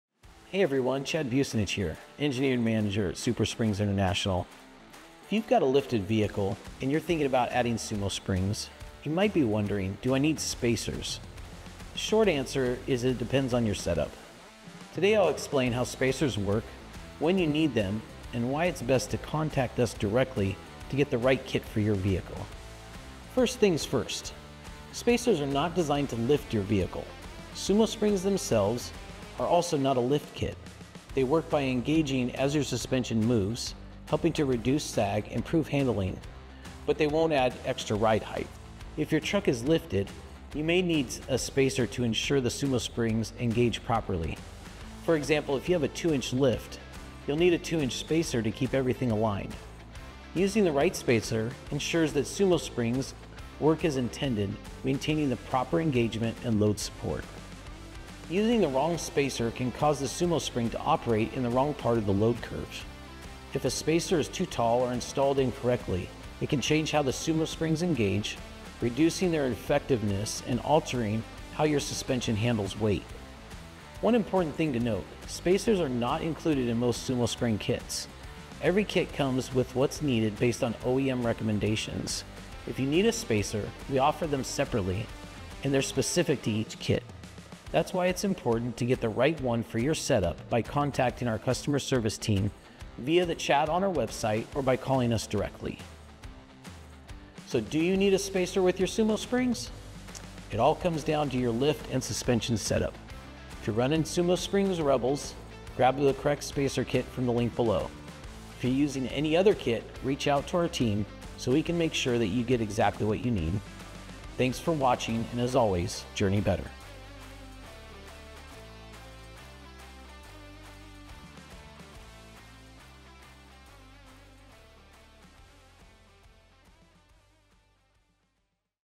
Do SumoSprings Need Spacers on Lifted Vehicles? | Engineering Corner
If you have a lifted vehicle and you’re thinking about adding SumoSprings, you might be wondering If you need spacers. In this video, Engineering Manager, Chad Bussanich, will explain how spacers work, when you need them and why it’s best to contact us directly to get the right kit for your vehicle.